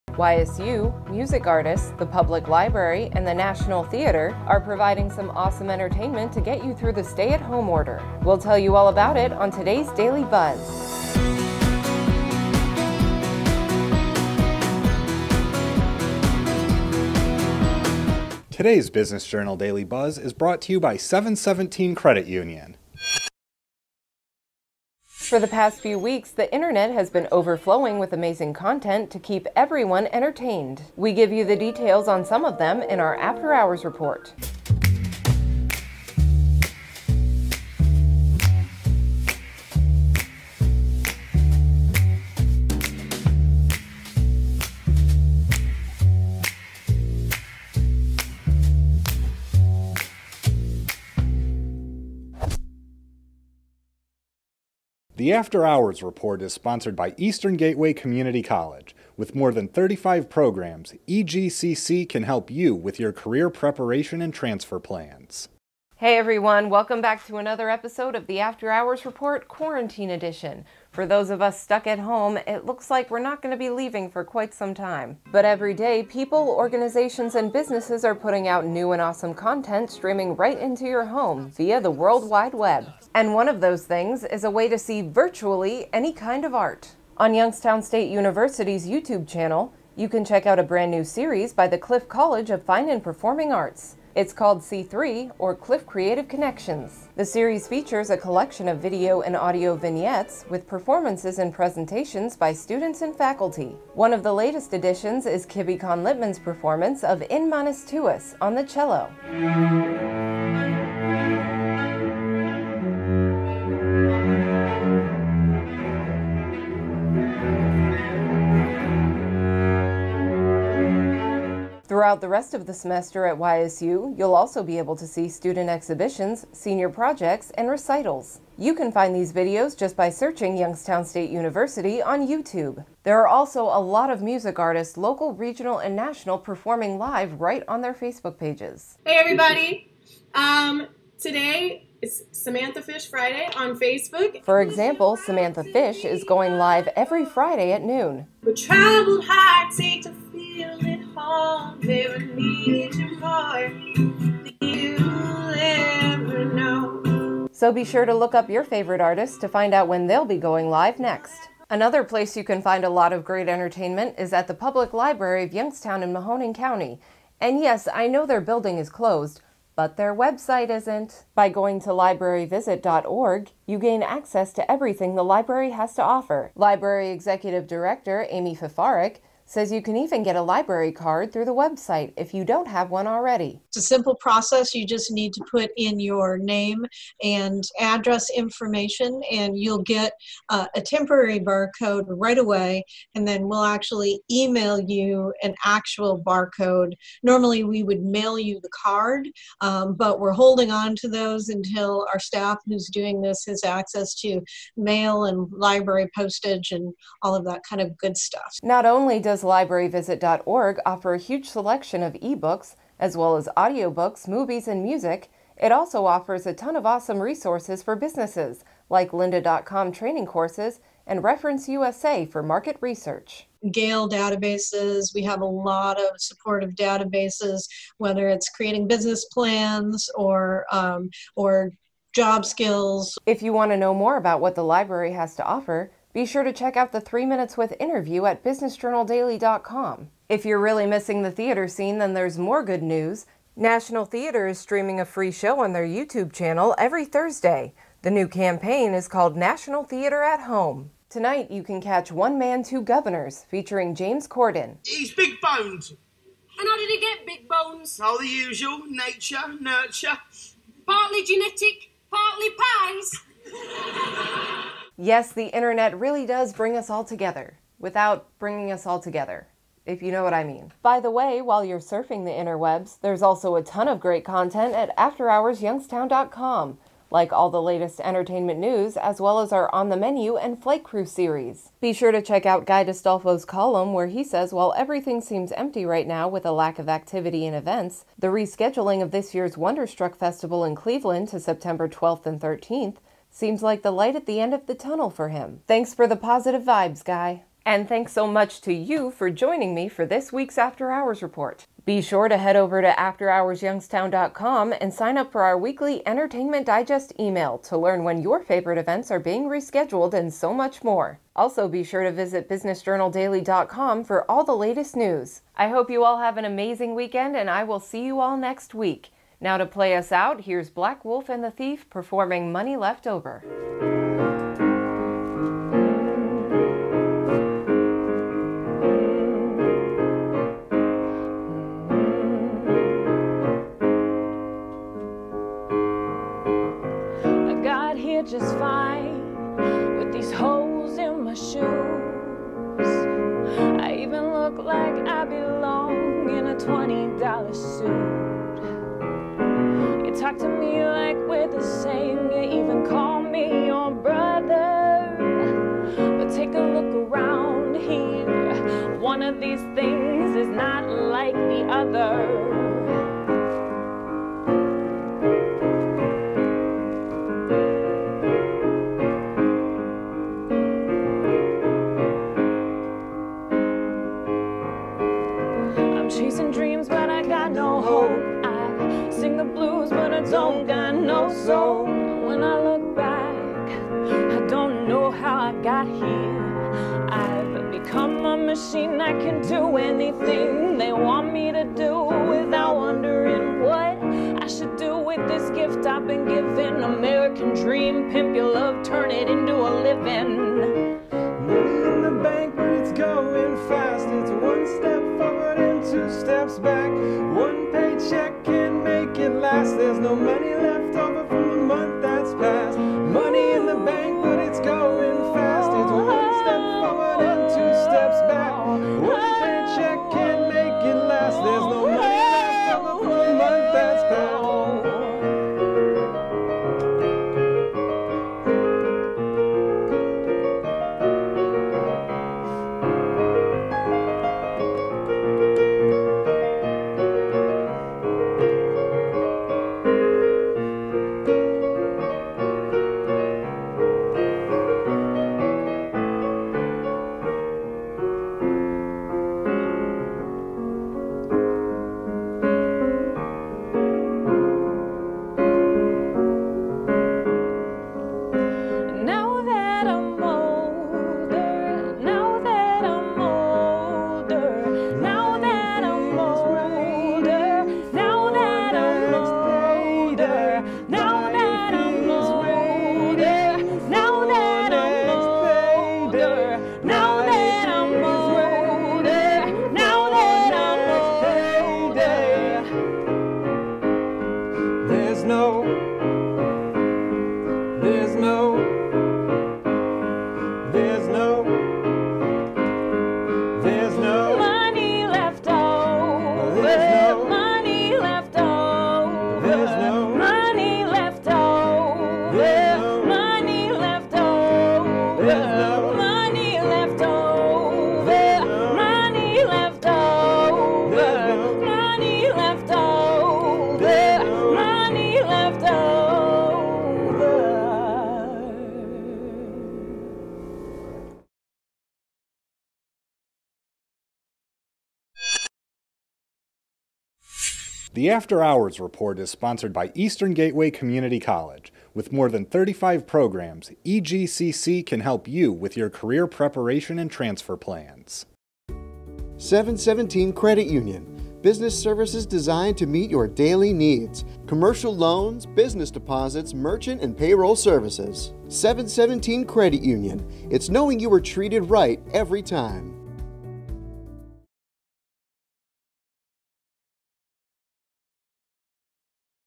An Internet of Entertainment | Daily Buzz 4-2-20 F
In the After Hours Report, we tell you about a variety of entertainment options available on the internet like music artists performing live on Facebook and National Theatre streaming shows on Youtube.